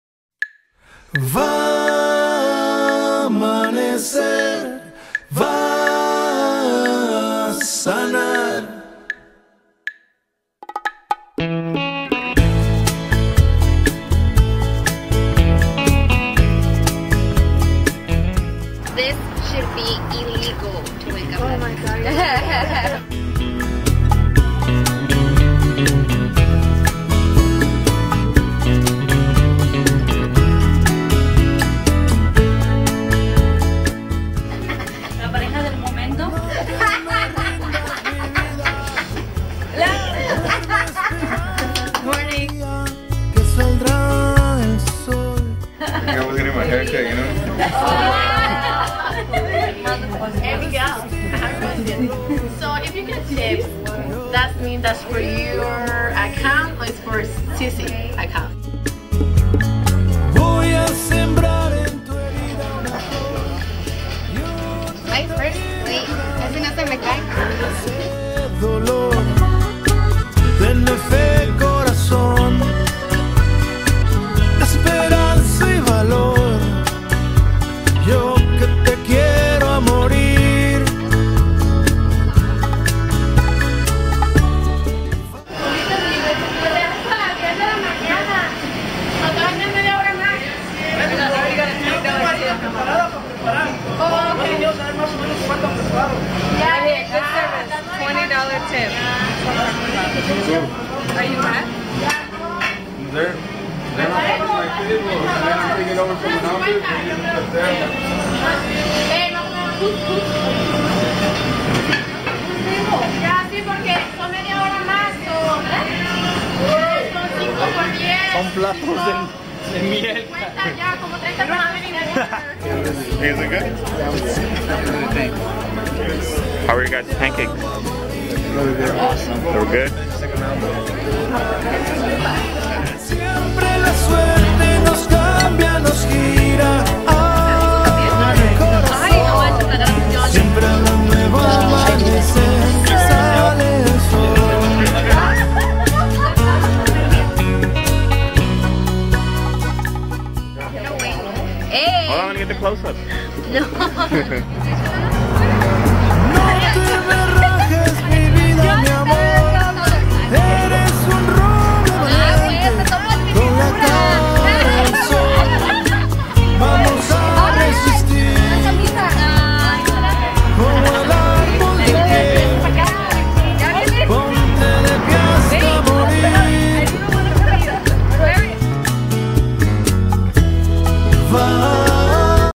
SJSU Chicano Commencement (Applebee's Fundraiser)
A look at SJSU Chicano Commencement Class of 2014's Applebee's breakfast fundraiser. 2/23/14